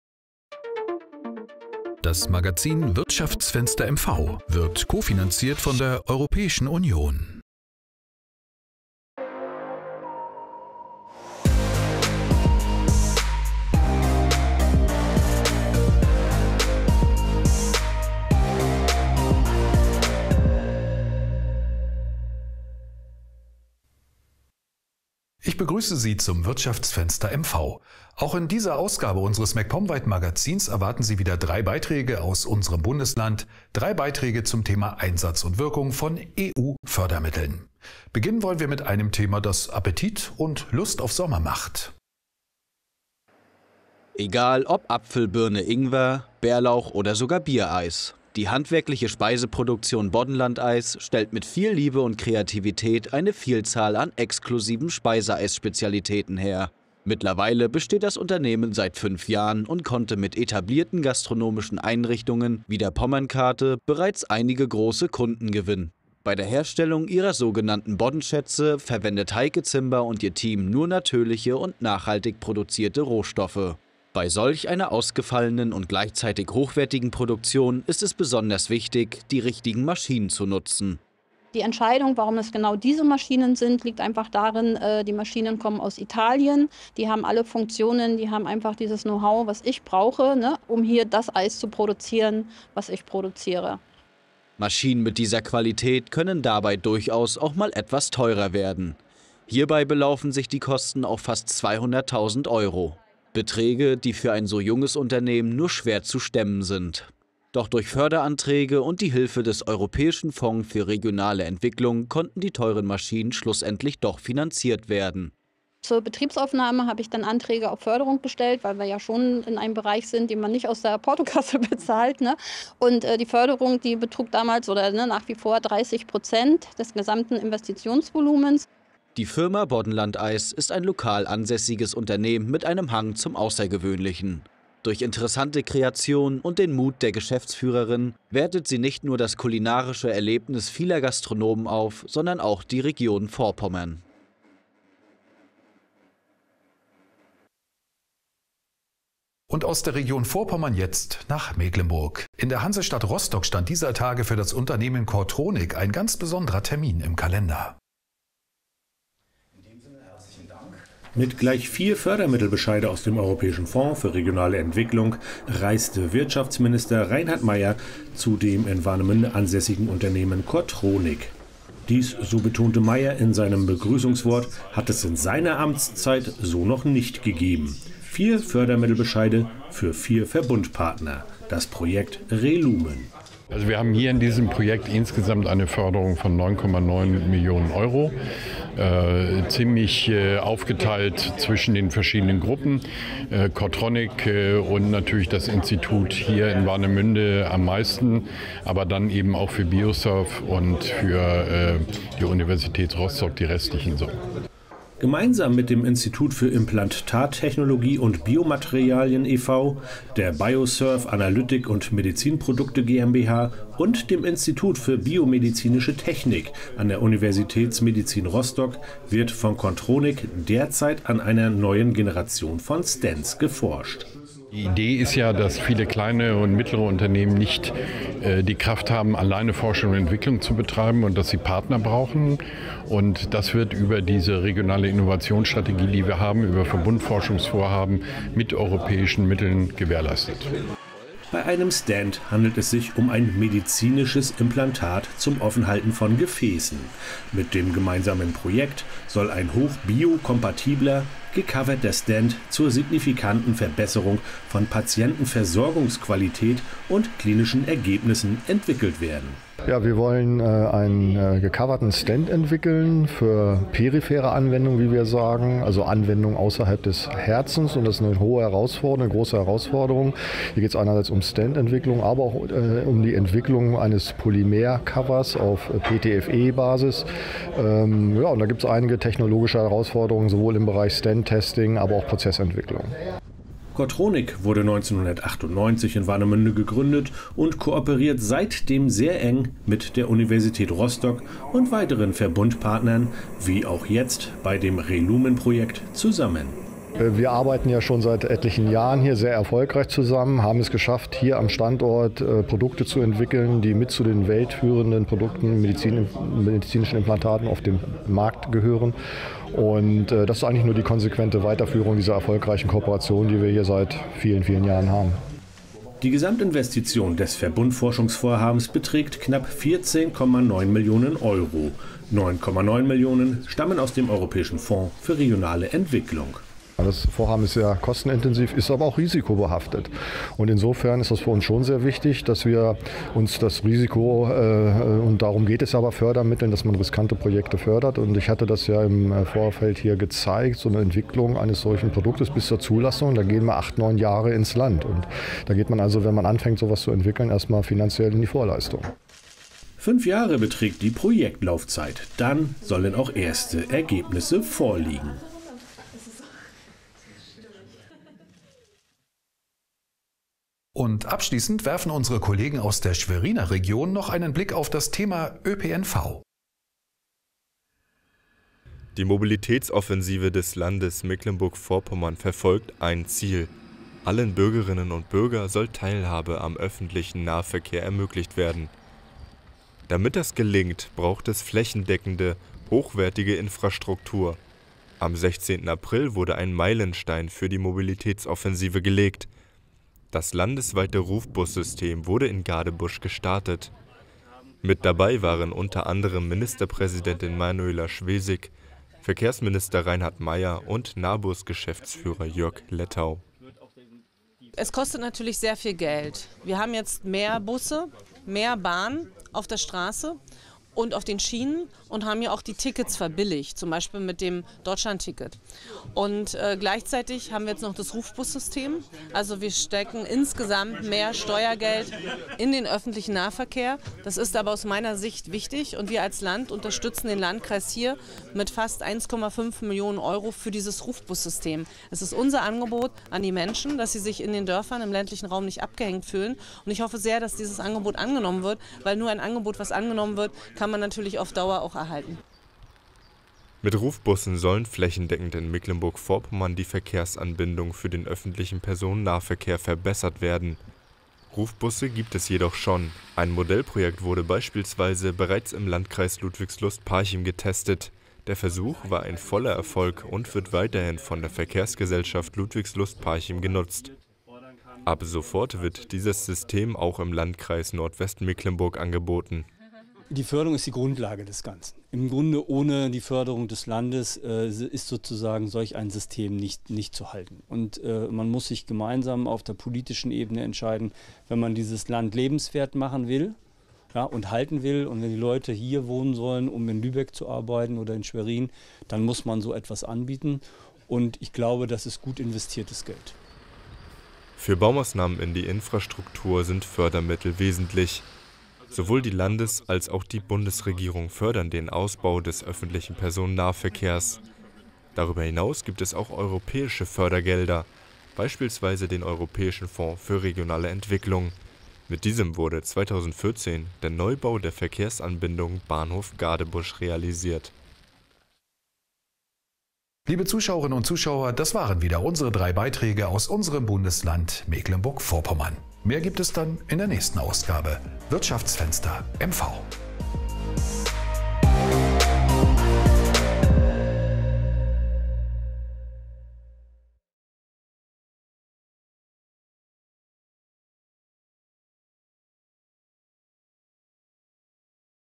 Flächendeckendes Rufbussystem für M-V
In Rambin bietet das Unternehmen Boddenlandeis ausgefallene Speiseeiskreaktioenen an. Ob Bärlauch, Rhababer oder Bier - die Eismaschinen machen's möglich. In Rostock unterstützt Finanzminister Meyer das Projket ReLumen. Die neue Mobilitätsoffensive MV beleuchtet das Rufbussystem in Mecklenburg-Vorpommern.